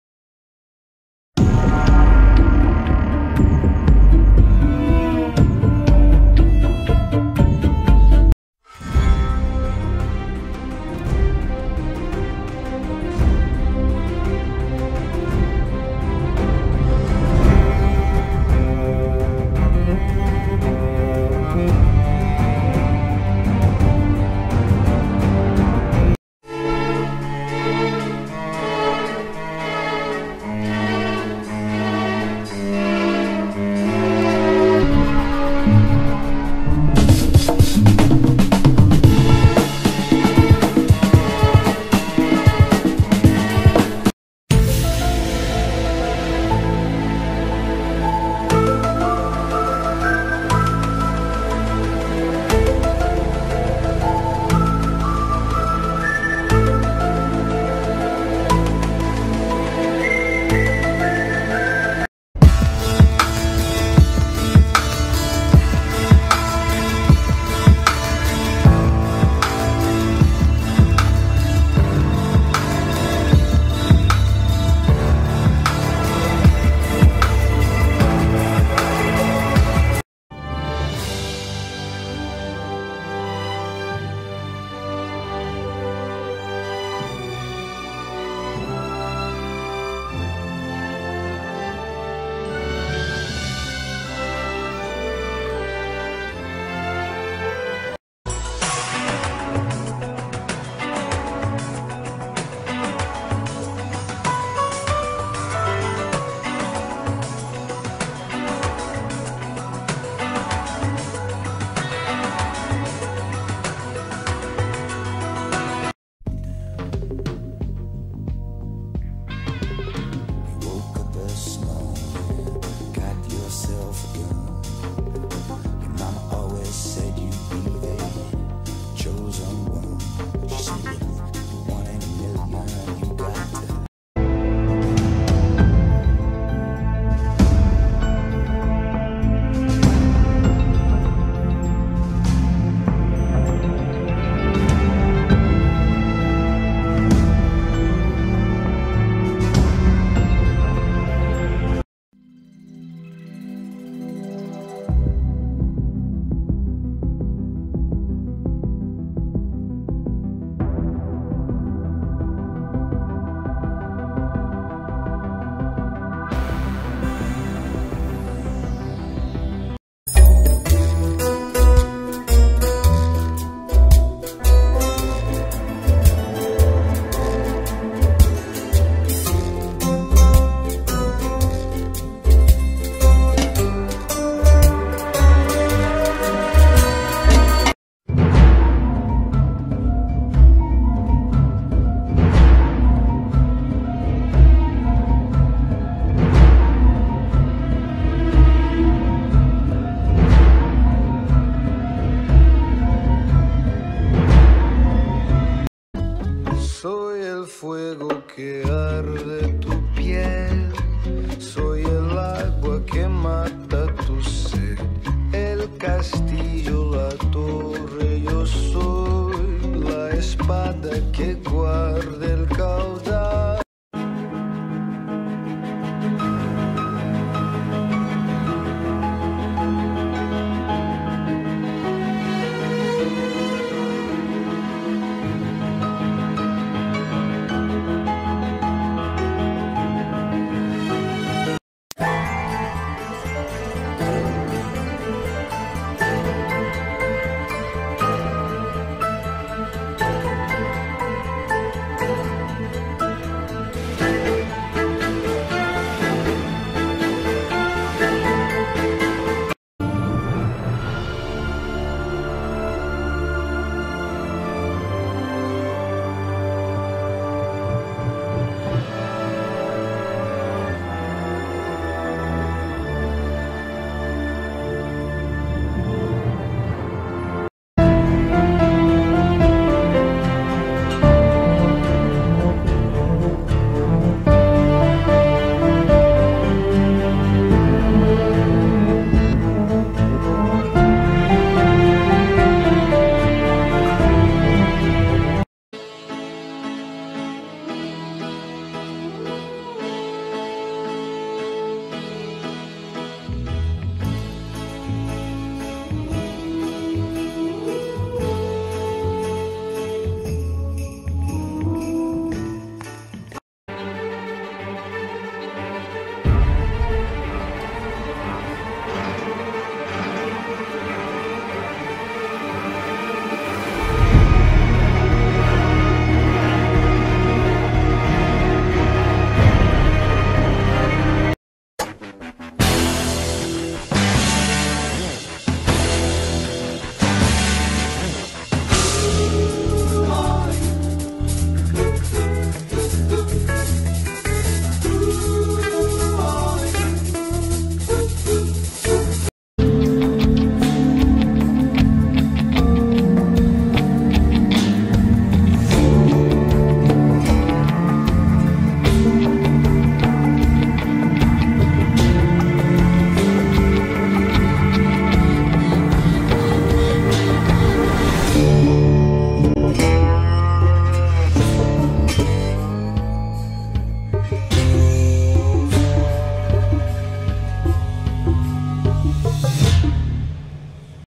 GUESS THE SCORE - DRAMA SERIES #1
15 seconds to guess the TV series theme. Can you win this challenge?